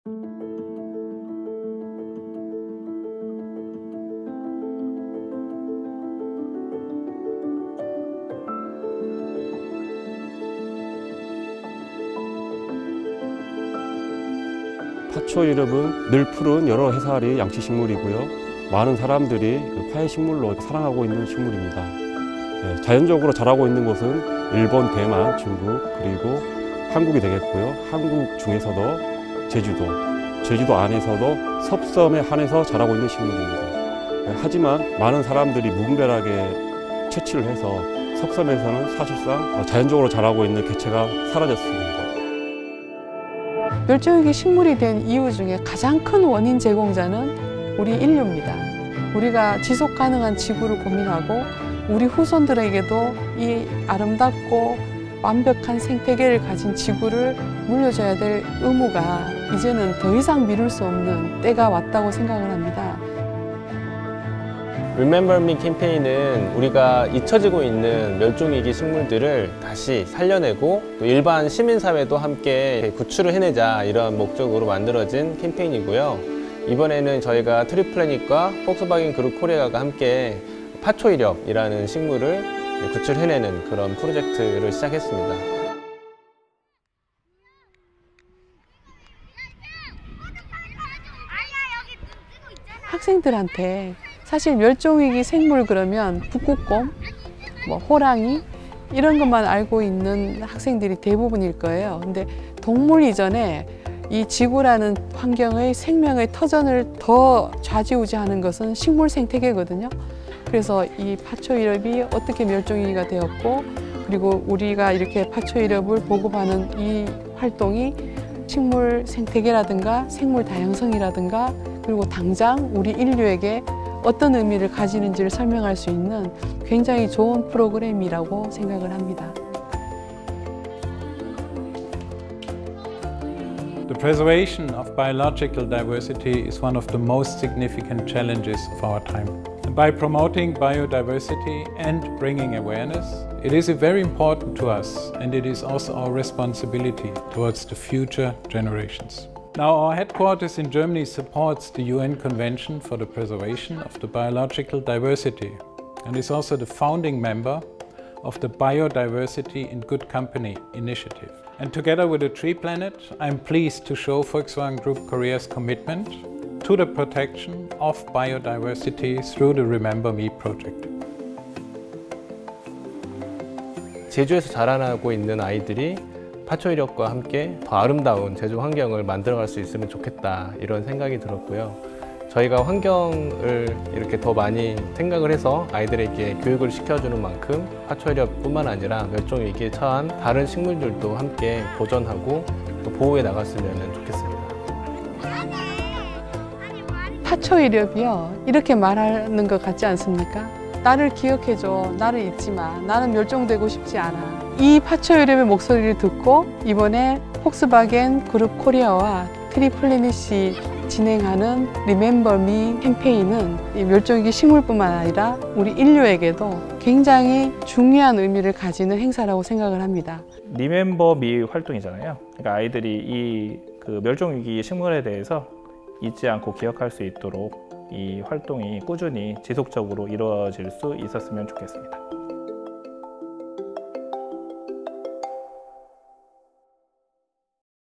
[Remember Me] 폭스바겐그룹코리아 2022 '리멤버 미' 캠페인
폭스바겐그룹코리아 는 소셜벤처 기업 '트리플래닛'과 멸종위기 식물들을 기억하기 위해 'Remember Me' 캠페인을 새롭게 시작했습니다.

'Remember Me' 캠페인의 첫번째 주인공은 제주도 섶섬에서 자라는 멸종위기 식물 '파초일엽'입니다.
폭스바겐그룹코리아와 트리플래닛은 제주 한림초등학교를 방문해 파초일엽을 기증하고, 환경 교육을 진행했는데요.
싱그럽고 푸릇푸릇했던 파초일엽과 학생들의 모습을 함께 살펴보실까요?